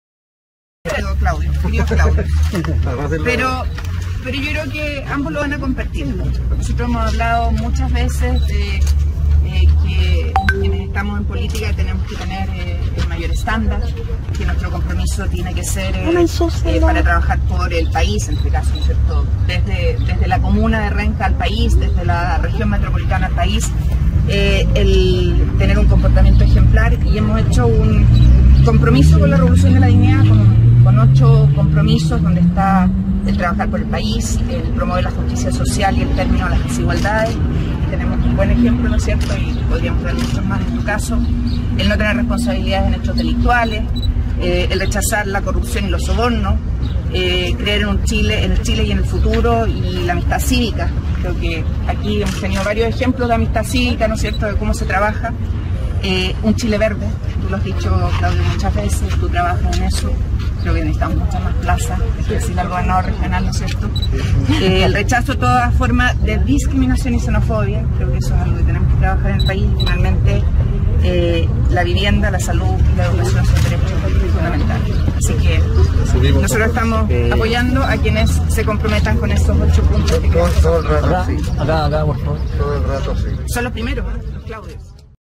Visita a la comuna de Renca, junto a Claudio Orrego y Claudio Castro.
La RevoluciónDeLaDignidad se construye sobre OchoCompromisos para tener un MejorPaís, más solidario, respetuoso, probo y consciente de cuidar su medio ambiente.
Gracias Claudio Orrego y Claudio Castro,  por ser los primeros en recibir estos compromisos y hacerlos realidad.